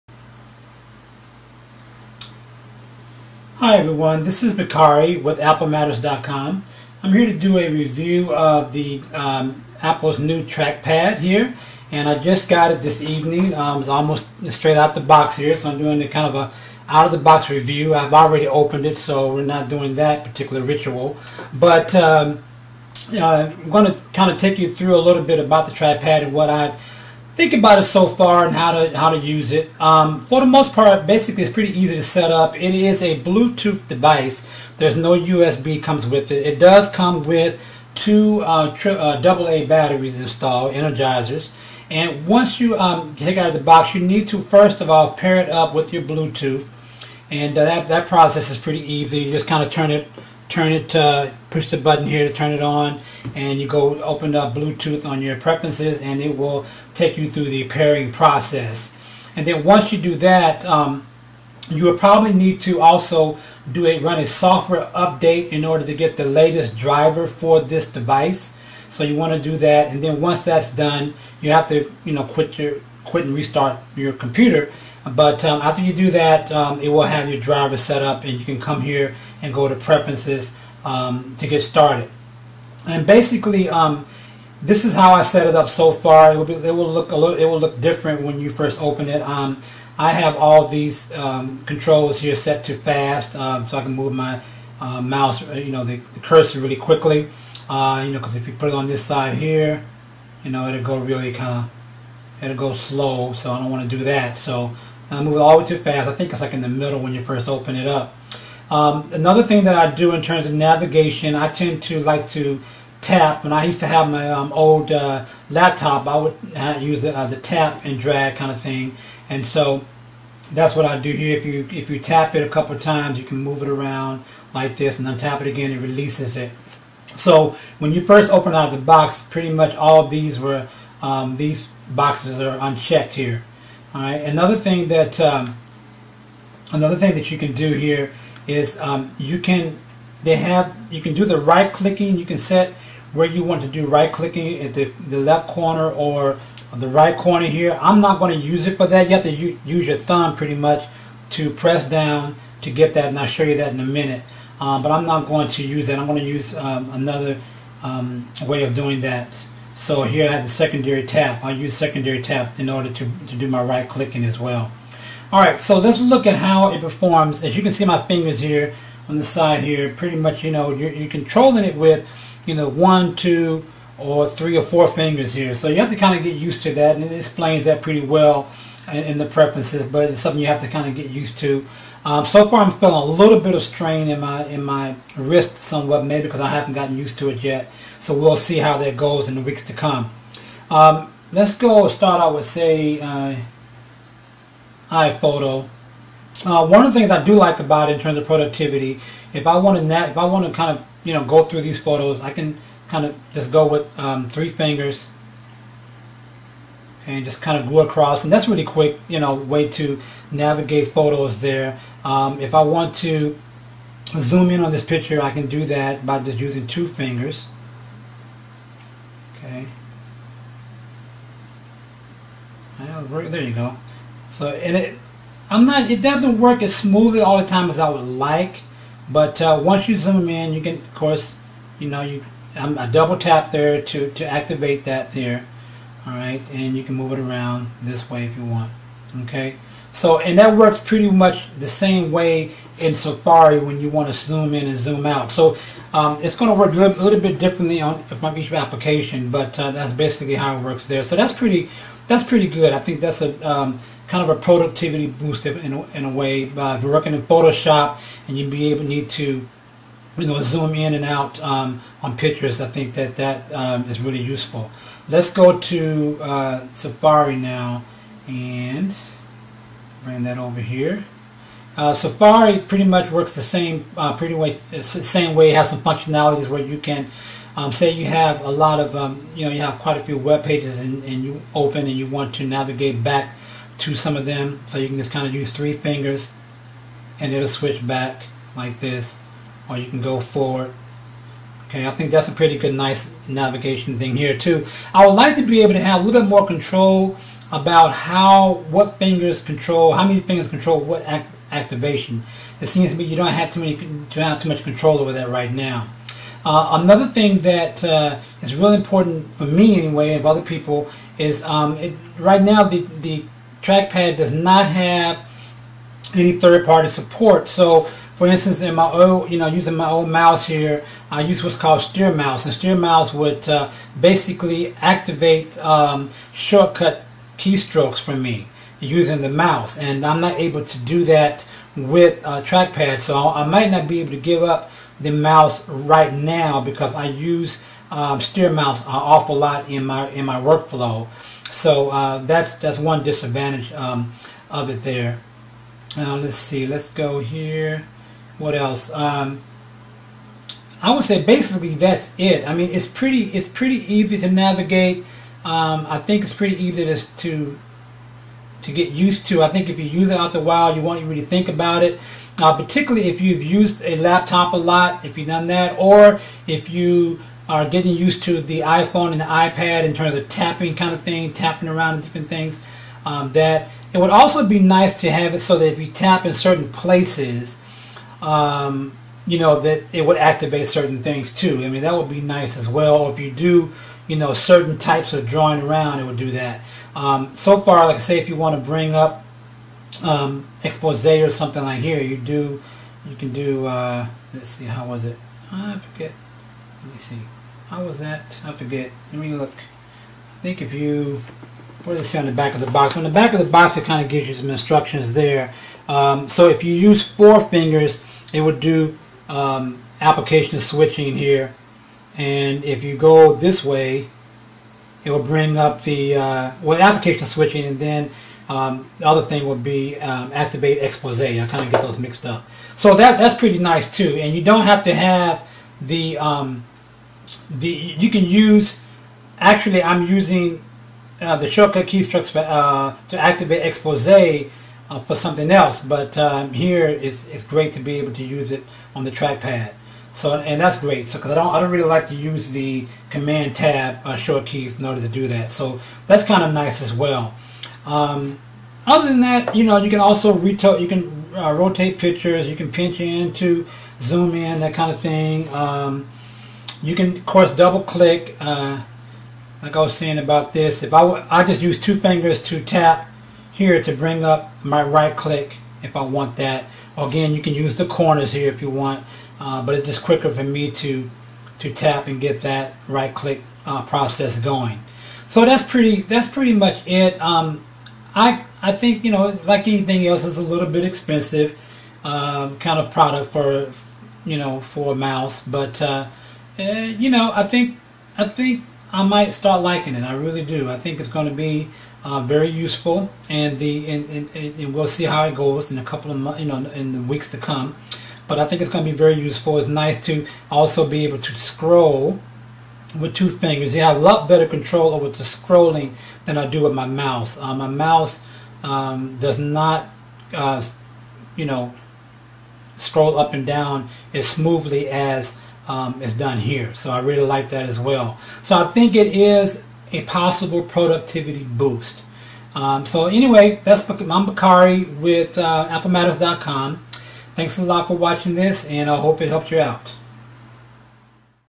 Overview of Apple's Magic Trackpad
Review and how-to for Apple's Magic Trackpad.